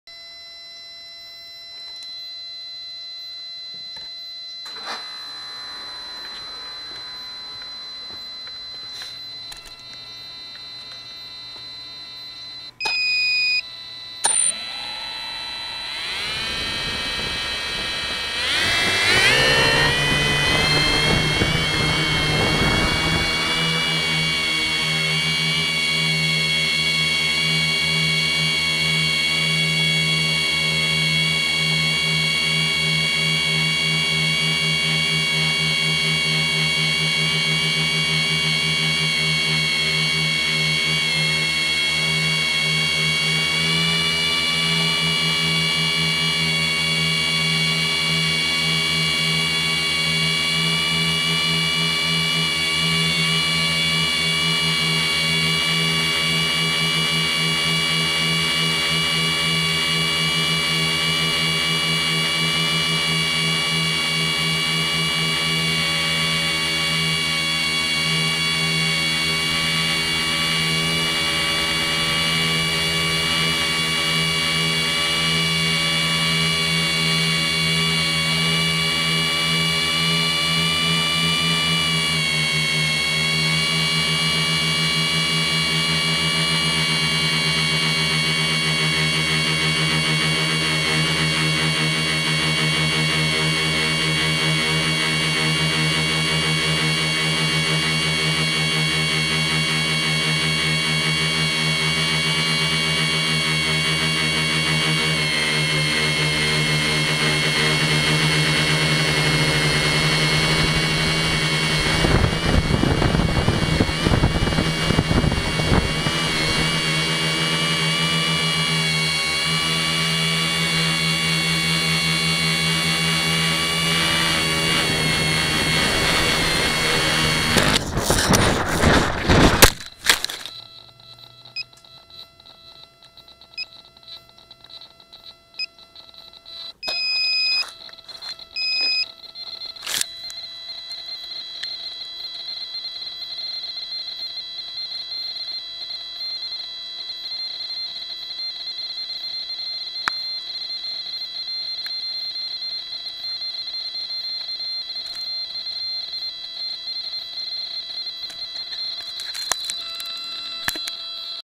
Mobius camera test---------- First Tricopter crash 2014
I was testing my new Mobius for fpv and bammmm right prop broke!
On impact with ground the two other props broke.